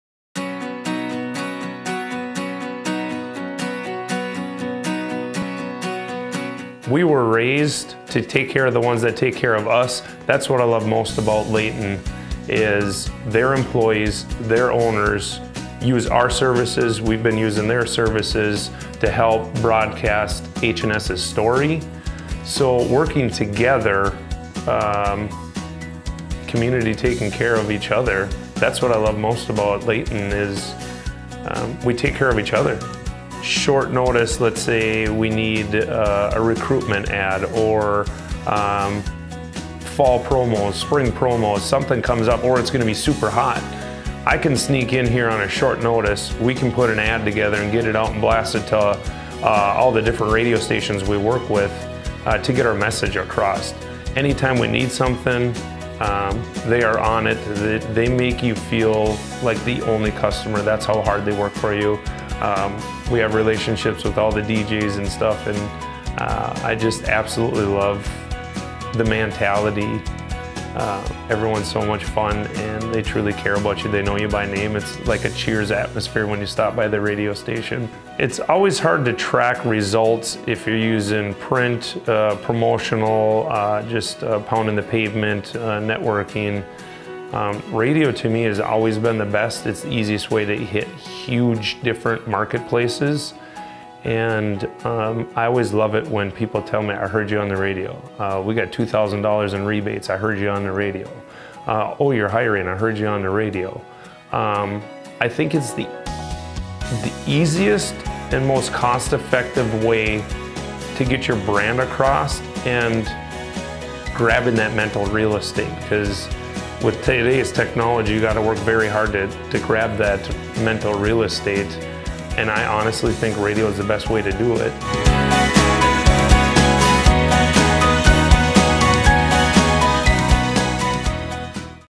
Leighton Media Client Testimonial - Jeremy Salzbrun, H&S Heating and AC
Hear from Jeremy Salzbrun of H&S Heating and AC about his experience with Leighton Media. His favorite aspect is the community taking care of one another.